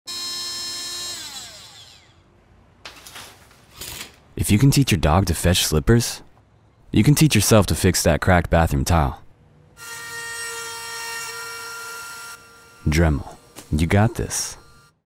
Du kan godt! med Dremel 💪
Gør-det-selv kan være lige så let som ”apport”! Hvis du kan lære din hund at hente hjemmesko, kan du også lære dig selv at reparere den revnede badeværelseflise. Når det kommer til at tackle boligreparationer, behøver du ikke at være ekspert. Du kan godt! med Dremel.

Hold dig informeret om nye Dremel-produkter, arrangementer, messer, specialtilbud, projekter og meget mere.